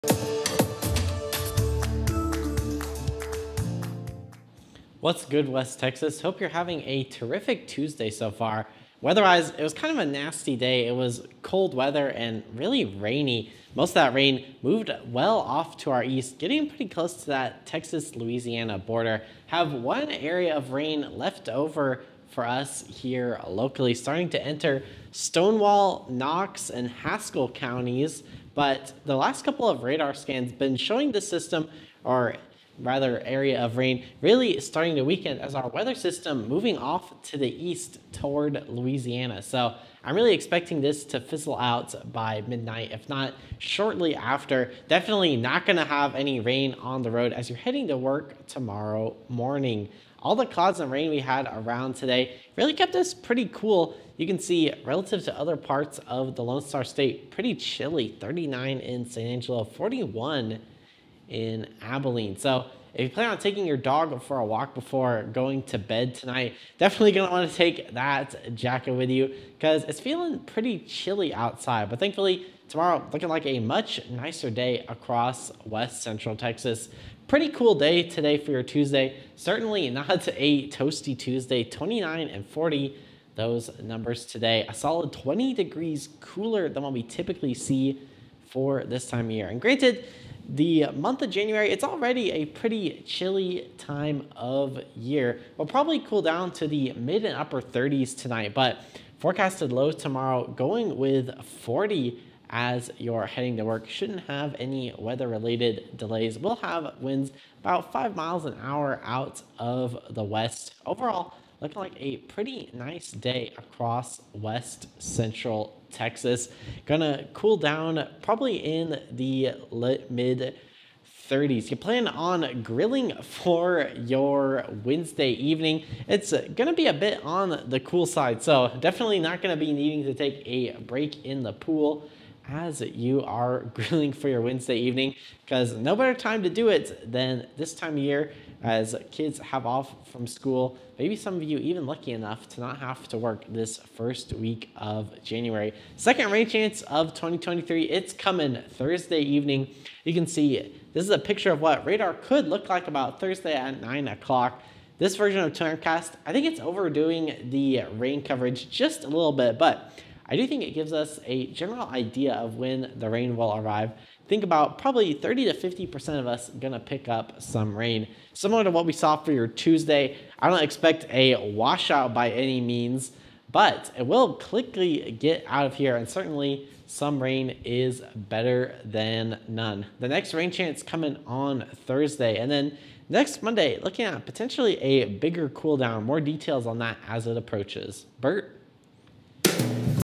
Jan. 2, 2023, night forecast
We are having a cold start to 2024, but we're monitoring next week for some much cooler air for West Texas.